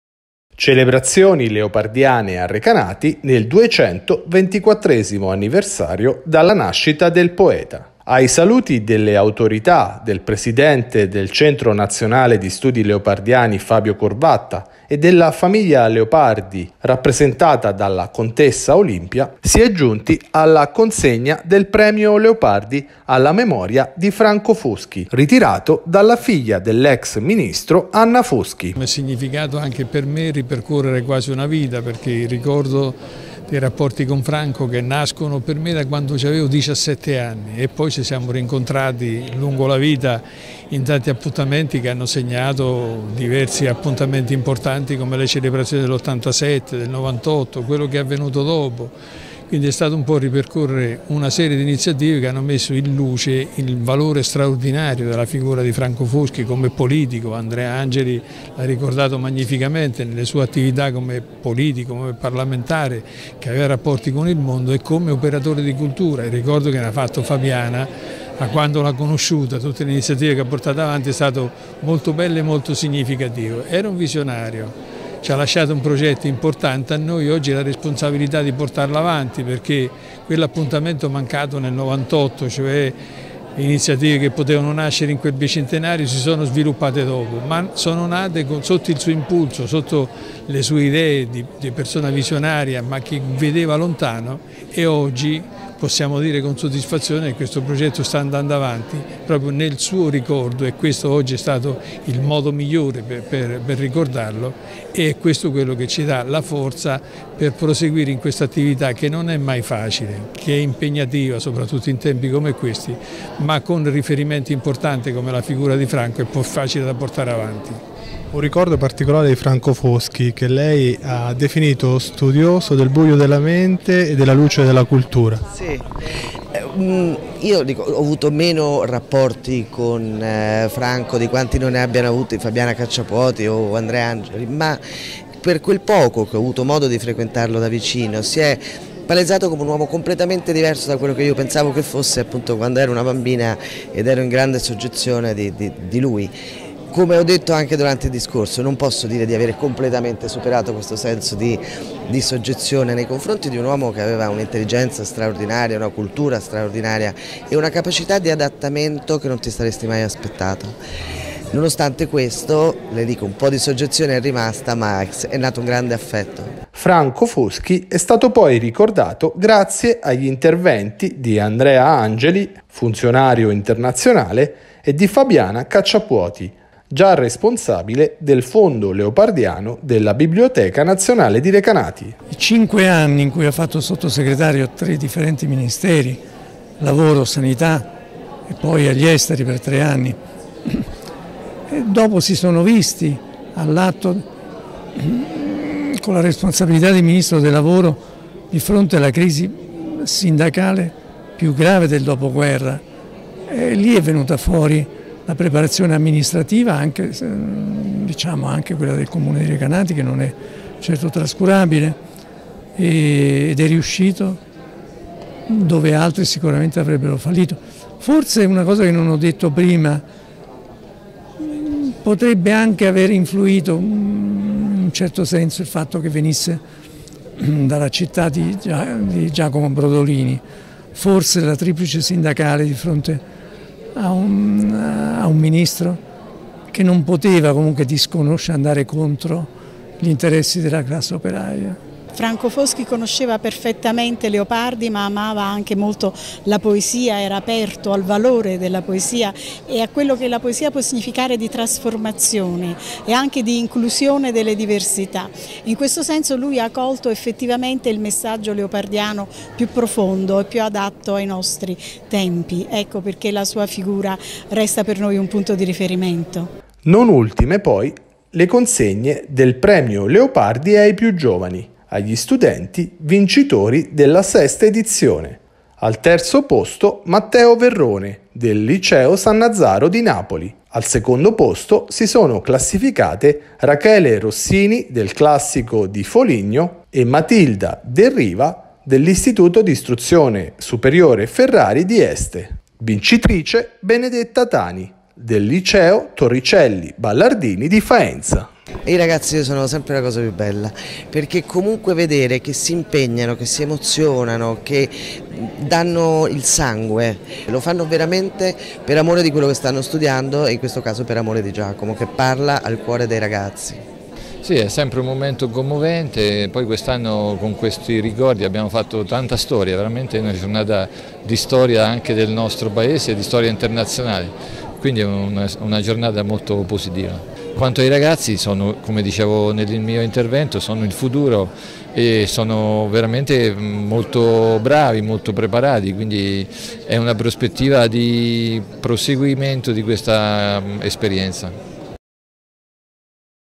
VIDEO TG. Consegnato il premio Leopardi alla memoria di Franco Foschi
Celebrazioni leopardiane a Recanati nel 224 anniversario dalla nascita di Giacomo Leopardi. Il premio Leopardi quest'anno è stato consegnato alla memoria di Franco Foschi. Vediamo...